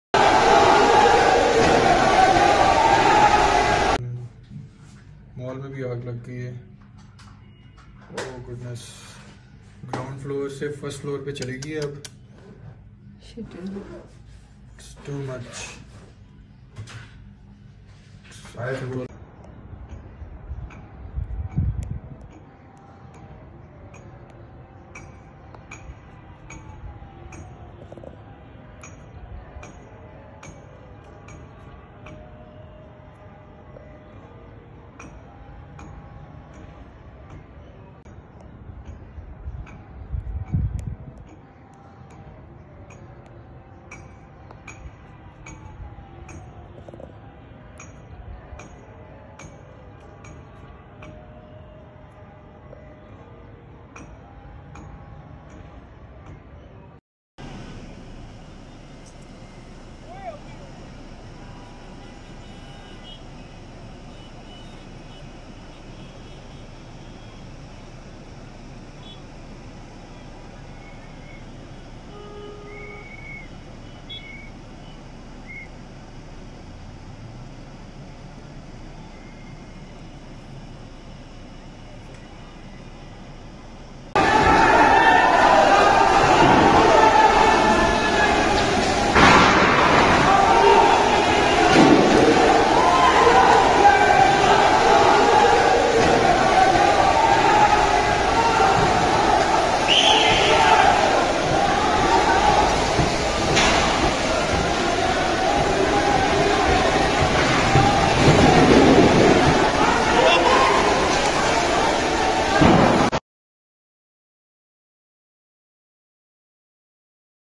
Fire in Centaurus Islamabad | Centaurs under fire | Fire Erupts Centaurus
Huge fire breaks out in Centaurus Mall in Islamabad
A huge fire broke out in Centaurus Mall in Islamabad, capital of Pakistan, on Today (Sunday-9-10-22)

According to initial reports, no casualties have been reported so far as the fire brigade rushed to the site of the incident to contain the flames.

Fire erupted on the third floor of the Centaurus Mall in Islamabad and is rapidly spreading across other floors.